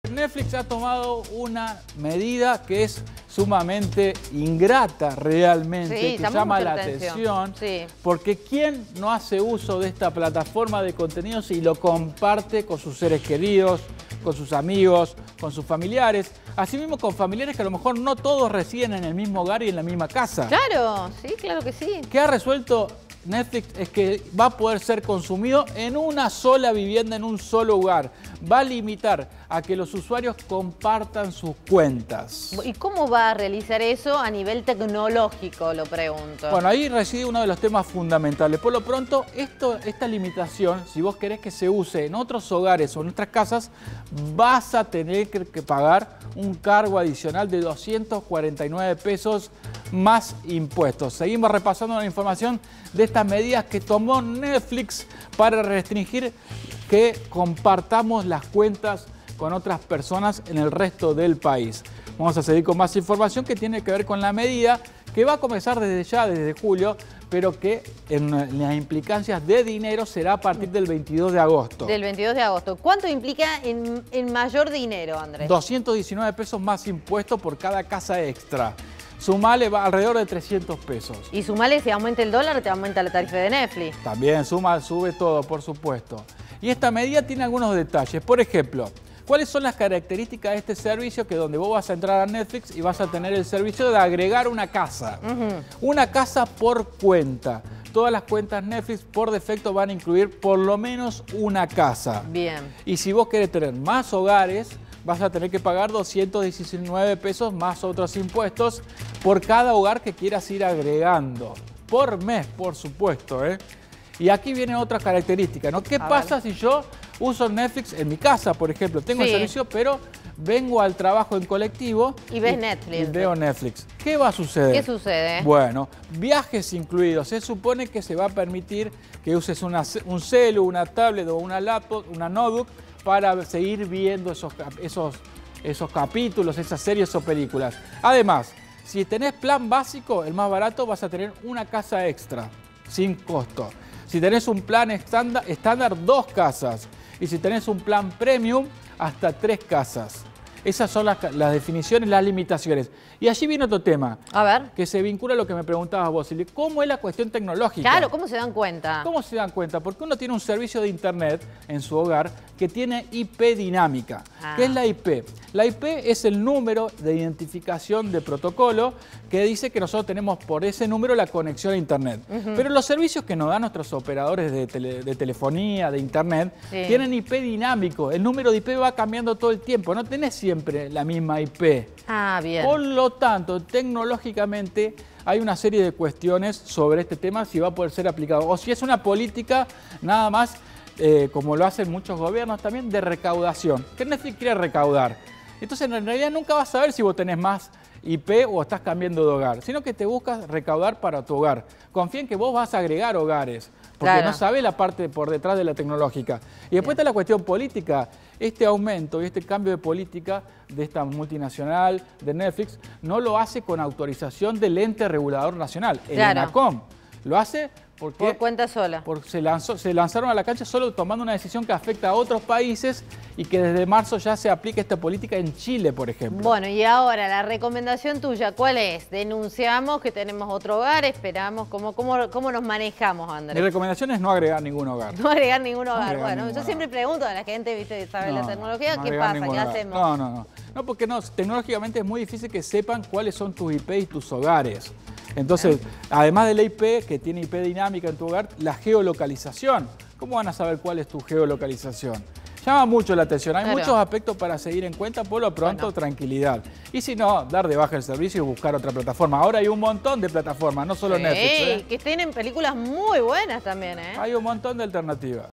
POLÉMICA MEDIDA RESTRICTIVA DE NETFLIX: Ya no se podrán compartir gratis las cuentas
Quienes quieran seguir compartiendo podrán hacerlo, siempre y cuando paguen un costo adicional de 219 pesos por hogar extra. La medida, que comenzará a regir el 22 de agosto.

¿Qué opinás de la medida resuelta por la plataforma?

COMPARTILO!